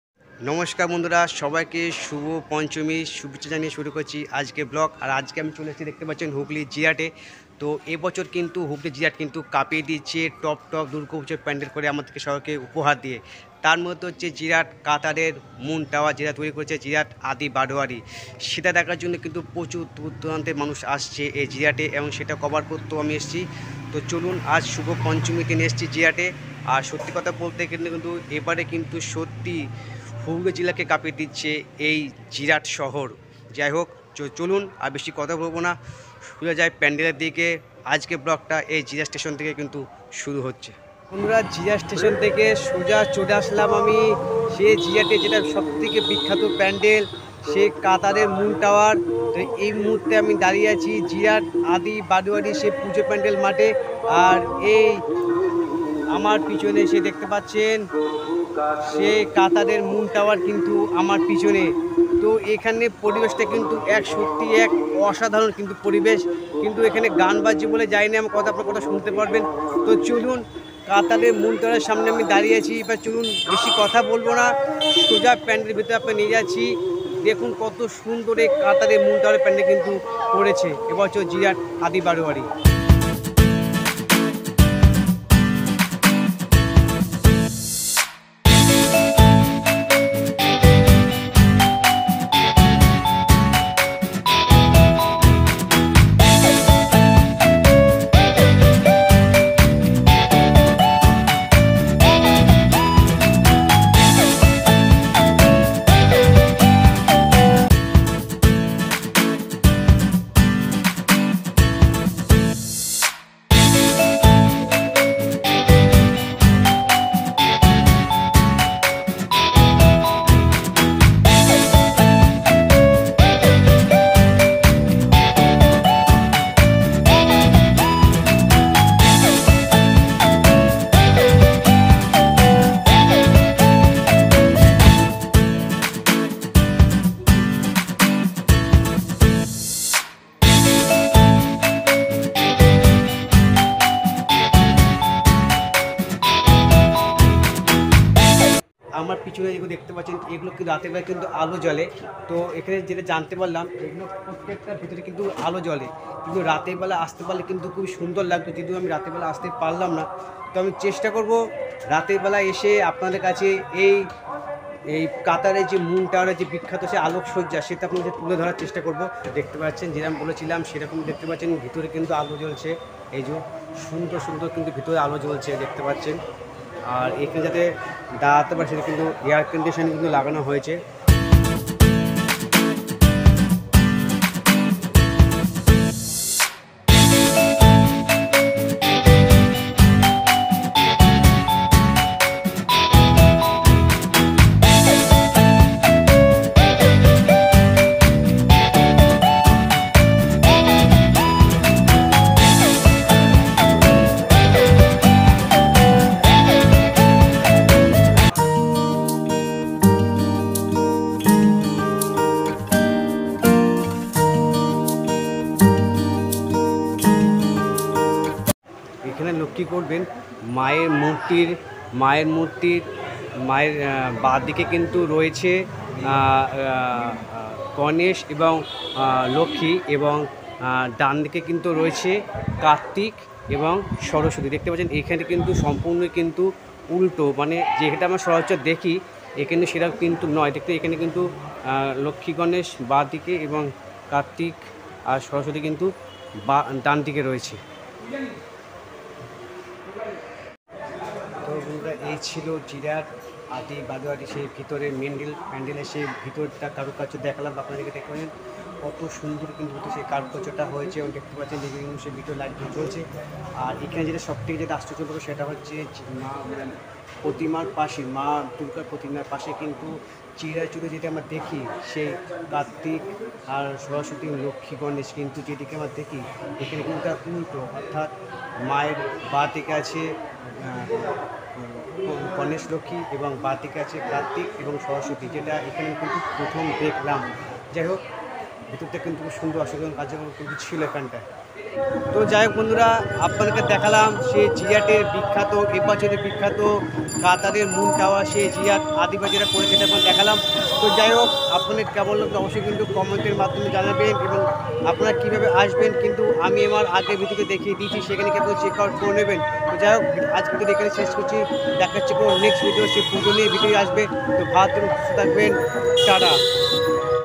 কাতারের মুন টাওয়ার || জিরাট দুর্গা পূজা || jirat durga puja 2024
কাতারের মুন টাওয়ার এই  বছর জিরাট আদি বারোয়ারি ক্লাবের মেন থিম সেই পুজোর প্যান্ডেলটি আপনাদের কাছে তুলে ধরা হয়েছে এই পুজা দেখতে হলে হাওড়া থেকে কাটোয়া লোকাল ট্রেন ধরে জিরাট ষ্টেশনে নামতে হবে

# jirat durga puja          # durga puja 2024